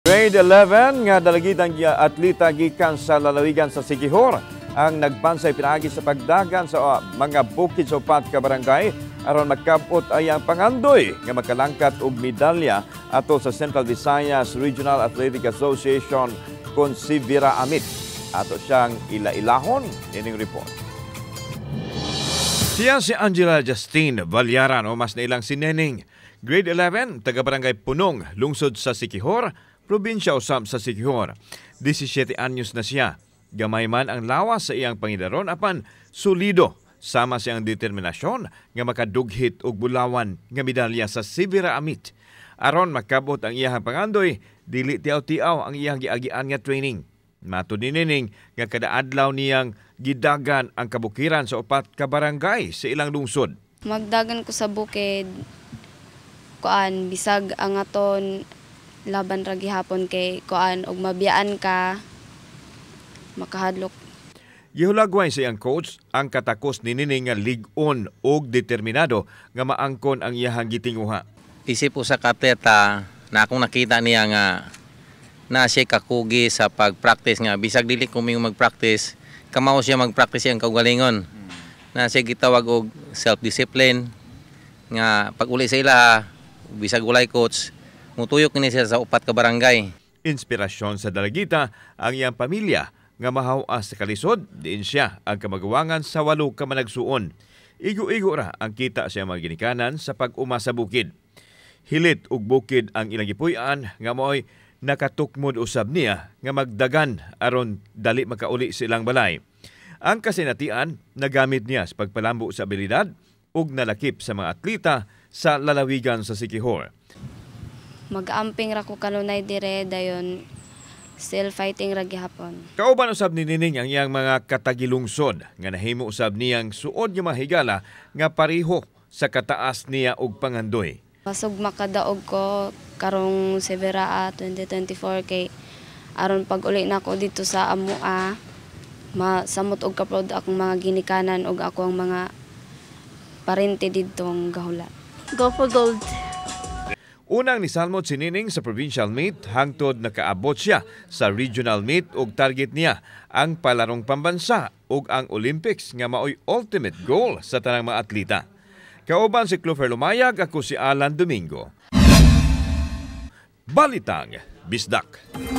Balitang Bisdak: Estorya sa pagpaningkamot sa atleta gikan sa Siquijor, sayran
Grade-11 nga dalagitang atleta gikan sa lalawigan sa Siquijor nagbansay pinaagi sa pagdagan sa bukid sa upat ka barangay aron makab-ot ang iyang pangandoy nga makalangkat og medalya sa Central Visayas Regional Athletic Association kon CVIRAA.